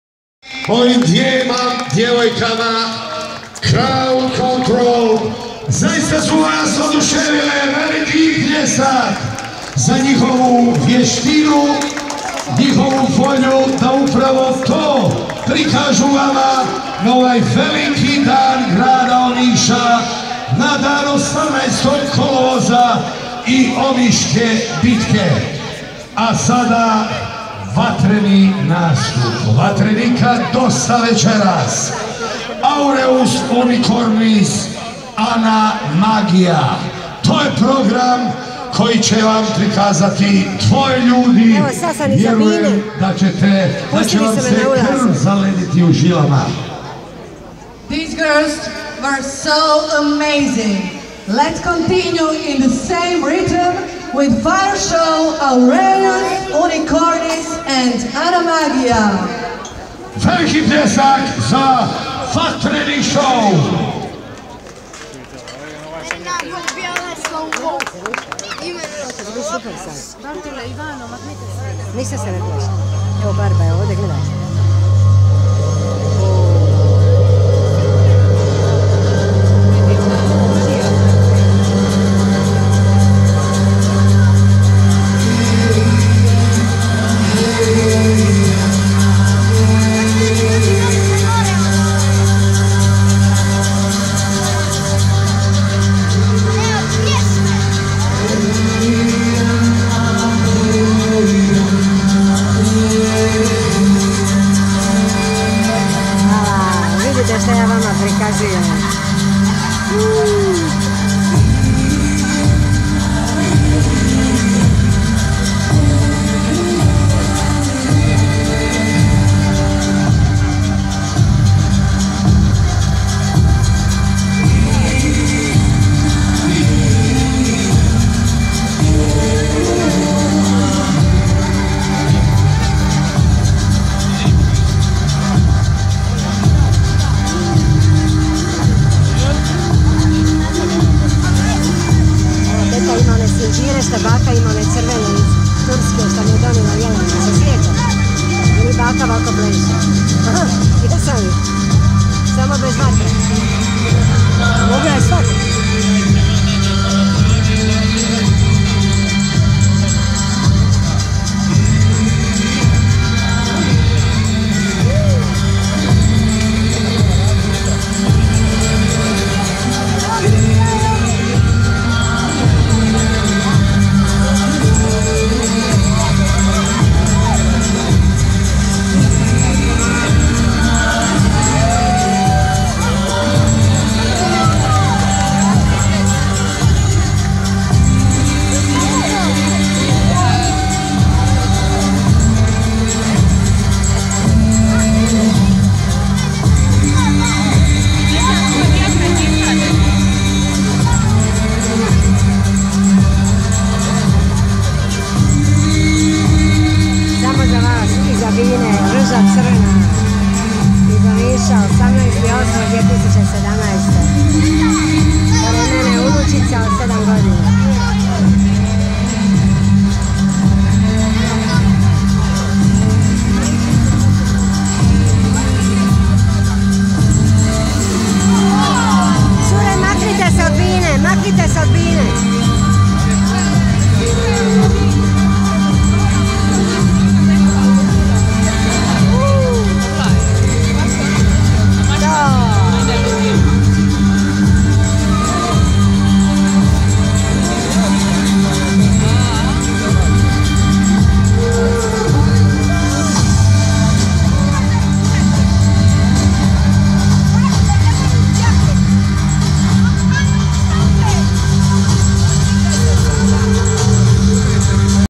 Gusarska bitka 2017, uvijek 18. 08 , Omiš, vrući dio 😉👍😀
Rekonstrukcija velike GUSARSKE BITKE i ove godine je bila besprijekorna , tisuće ljudi. mislim kao nikad do sada😀